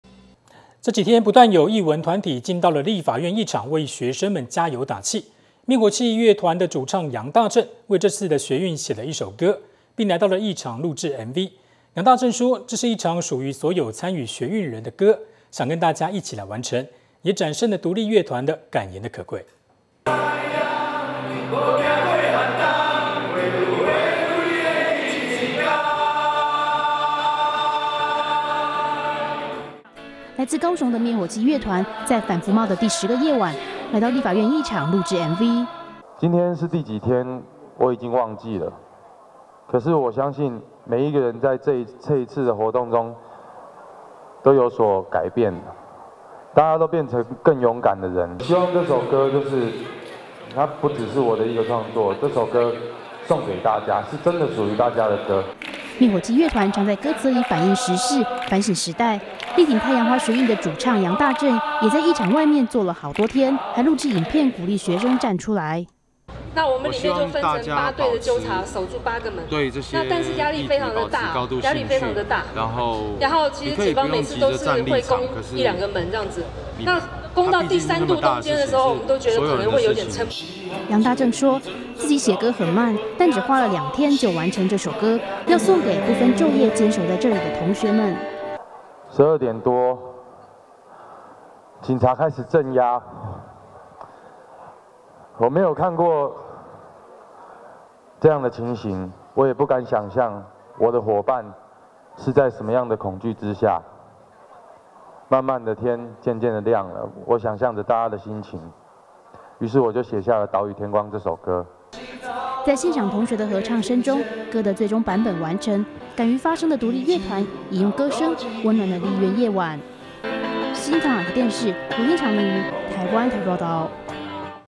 【台灣新聞】一首屬於大家的歌：滅火器議場拍MV |反服貿|318學潮|太陽花
【台灣新聞】一首屬於大家的歌：滅火器議場拍MV (NTDAPTV)